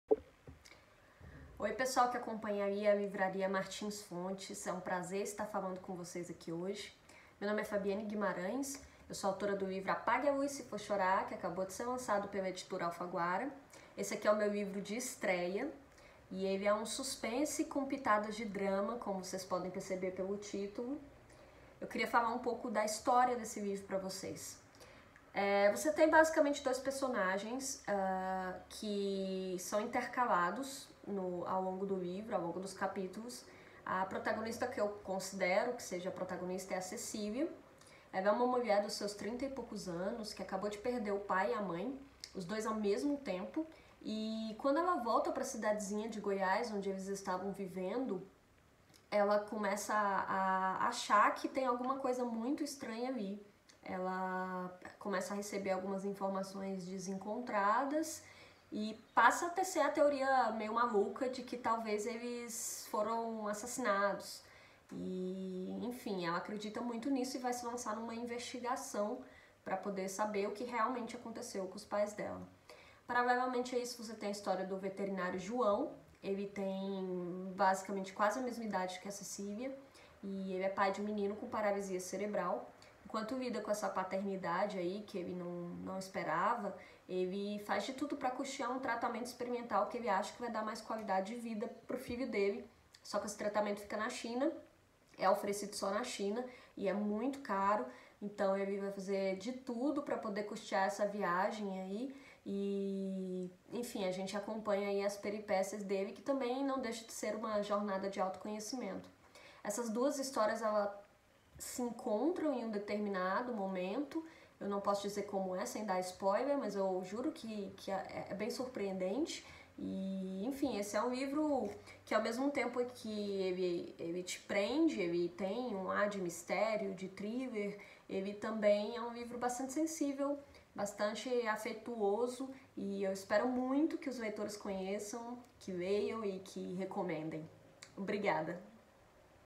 Com a palavra, Fabiane Guimarães!
Neste vídeo, a autora @fabicguimaraes comenta seu romance de estreia  "Apague a Luz Se For Chorar", publicado pela @editora_alfaguara.